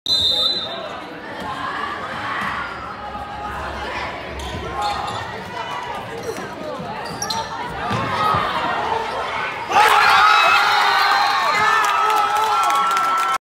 Williamston Middle School Basketball, Game Winning Shot
The 8th-grade Williamston Middle School Basketball team used a play familiar to college basketball fans to win at the buzzer vs. Haslett Middle School, December 2019. Go Hornets!